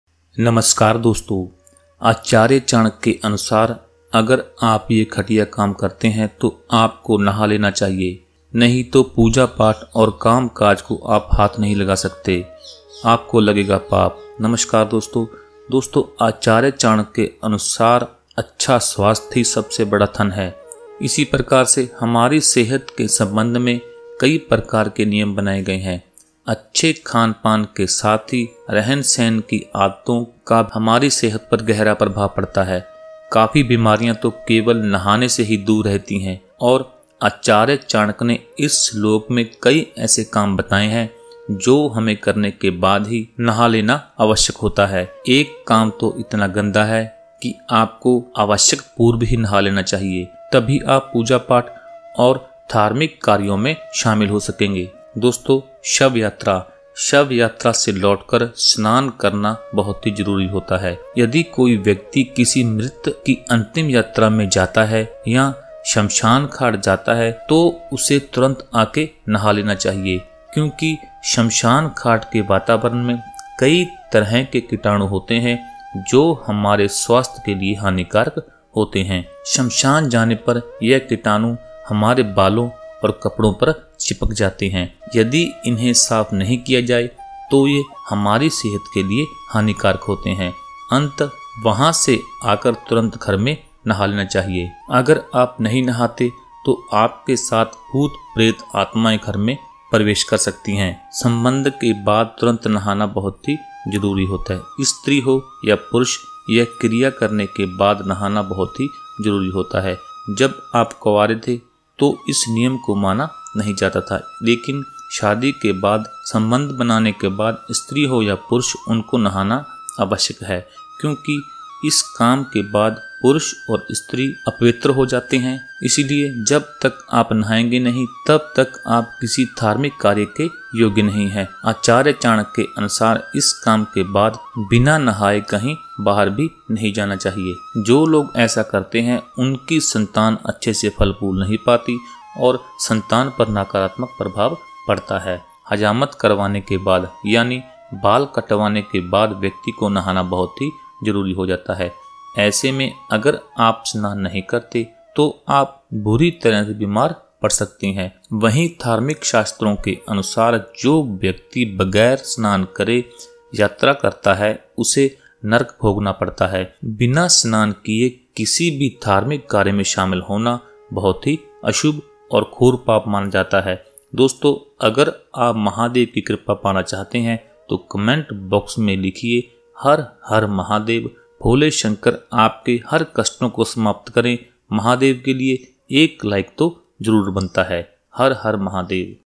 इन तीन कार्यों के बाद तुरंत करना चाहिए स्नान, वरना लगेगा पाप चाणक्य नीति
आचार्य चाणक्य के अनुसार, कुछ कार्य ऐसे होते हैं जिन्हें करने के तुरंत बाद नहा लेना ठीक रहता है. अगर आप इन कार्यों को करने के बाद तुरंत नहीं स्नान करते हैं तो कुछ परेशानियां हो सकती हैं. आचार्य चाणक्य के अनुसार, शमशान घाट से आने के बाद इंसान को तुरंत स्नान कर लेना चाहिए